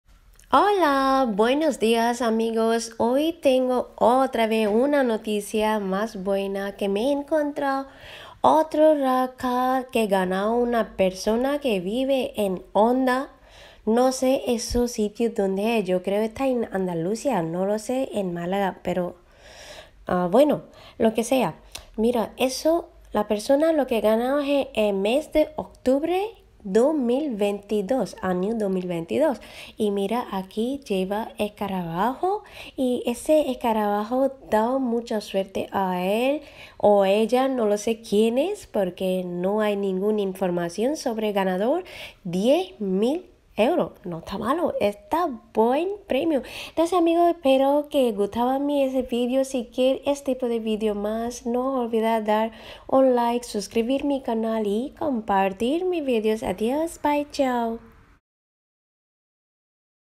CLEOPATRA - BINGO JACKPOT 🎰 - NUEVO Rasca de la Once 😱💥🎉🍾
En Onda, un nuevo rasca de Cleopatra de la ONCE premiado con 10.000 euros en mes de octubre 2022